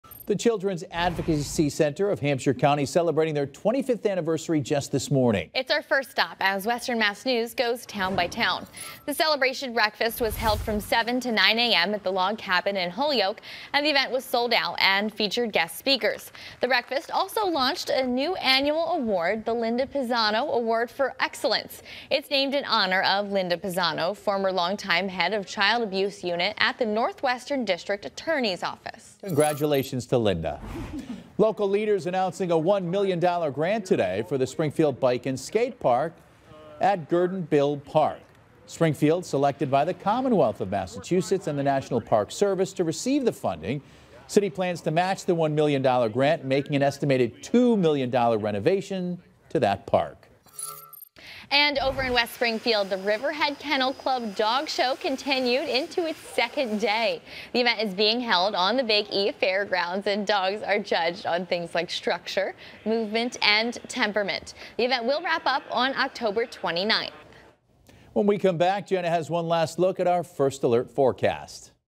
Town by Town: Children’s Advocacy Center celebration, Gurdon Bill Park grant, dog show
Town by town is taking you to Holyoke, Springfield and West Springfield.